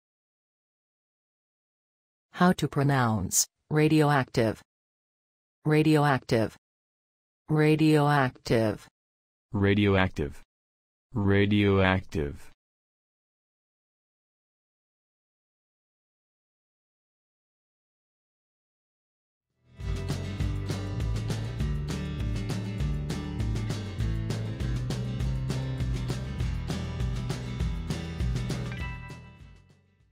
Radioactive pronunciation
How to pronounce Radioactive in English? Listen with us.
What is the correct pronunciation of the word Radioactive in everyday English? Speak as the Americans.
How to write the word Radioactive? Watch and learn.

Learn how to pronounce Radioactive.